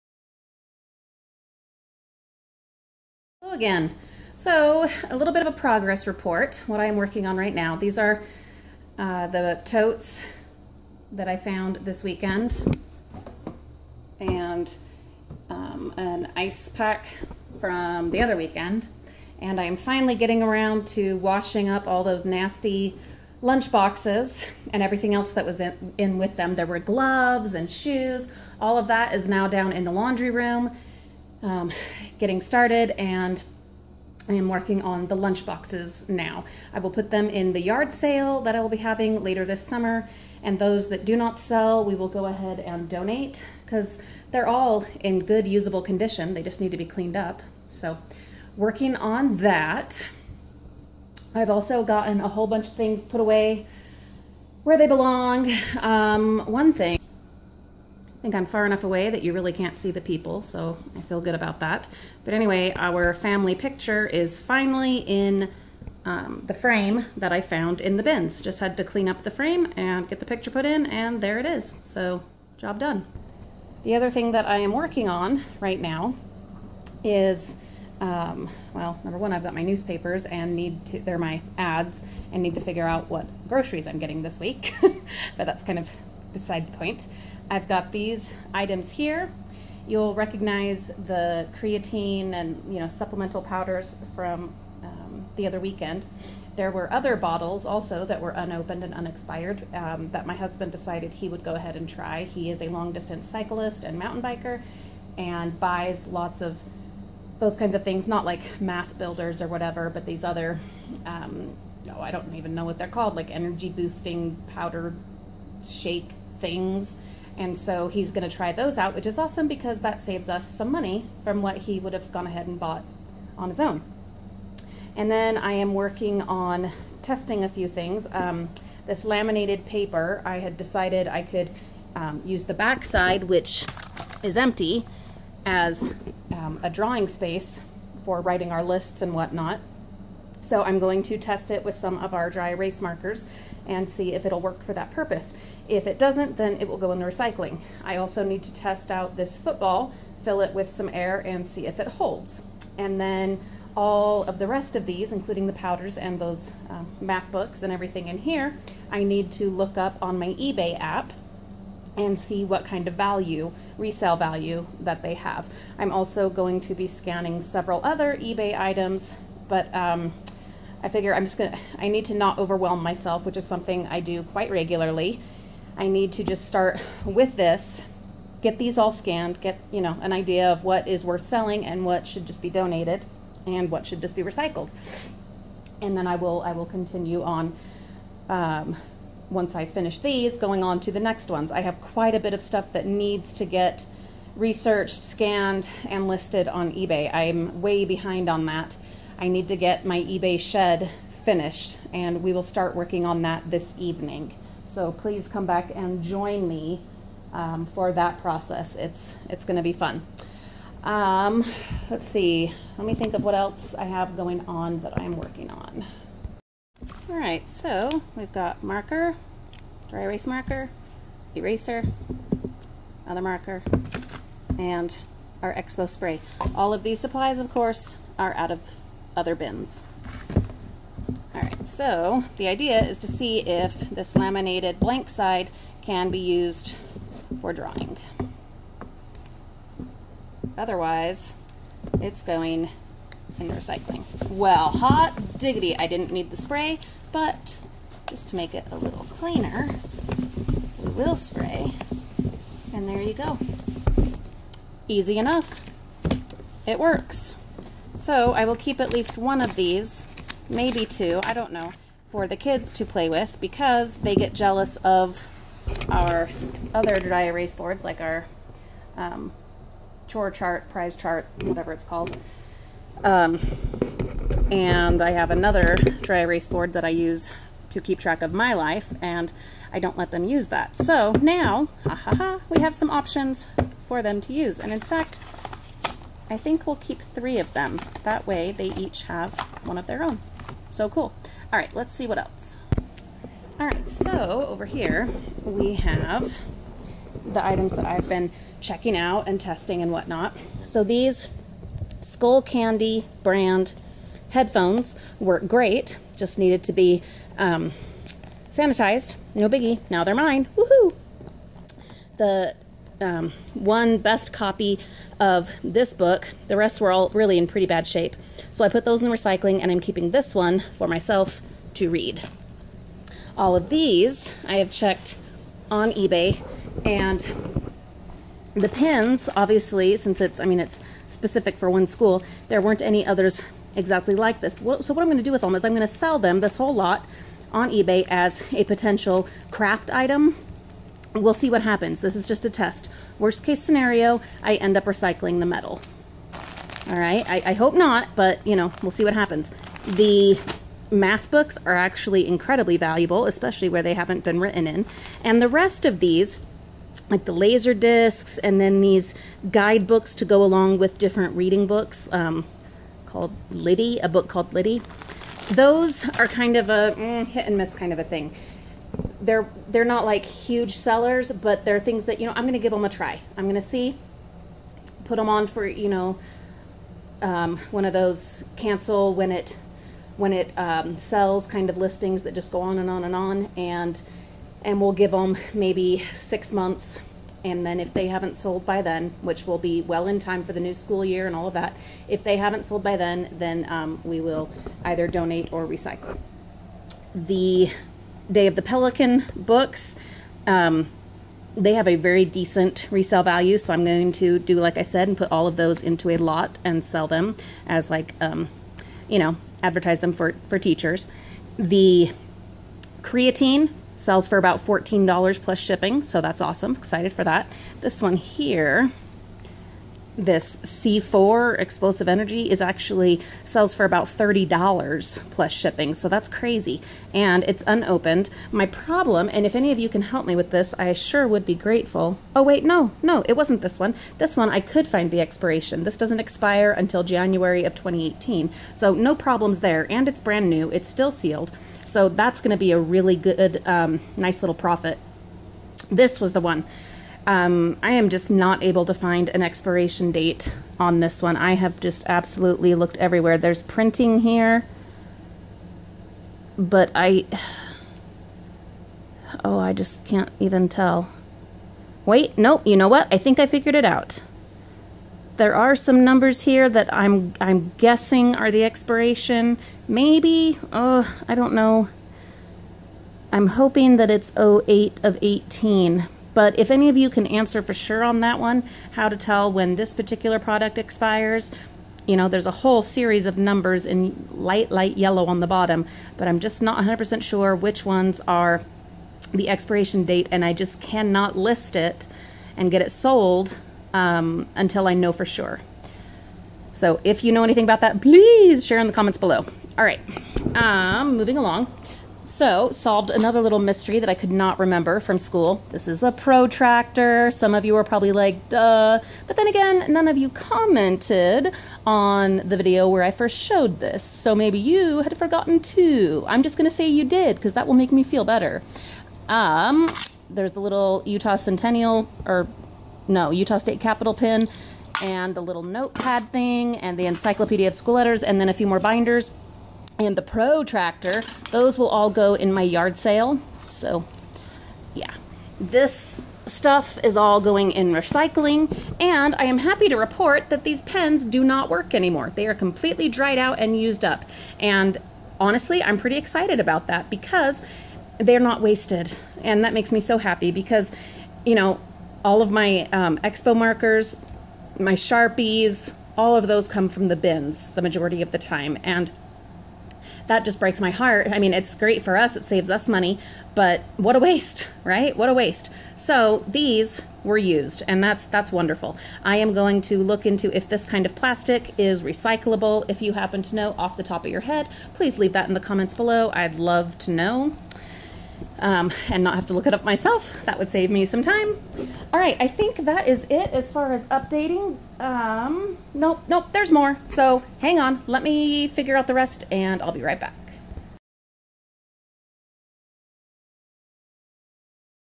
Dumpster Diving 2017: Progress Report! $$$$$ PART 1
I've been busy! Doing research, testing our discoveries, being organized! I see $$$$ in our future! :-)